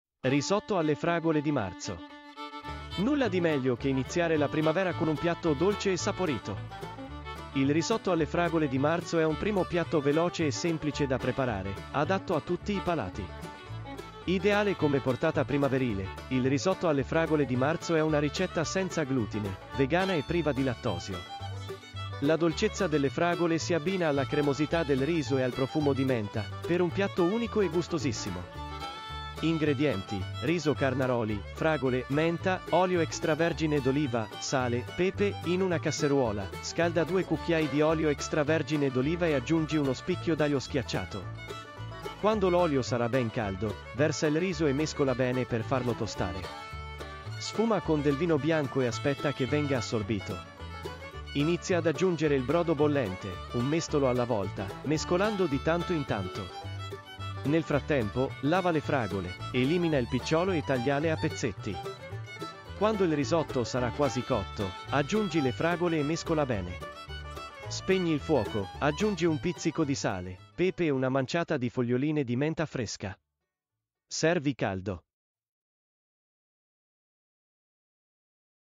Risotto alle fragole di Marzo
Inizia la primavera con un risotto dolce e saporito! Il risotto alle fragole di Marzo è un primo piatto veloce e semplice da preparare, adatto a tutti i palati. - Scopri di più su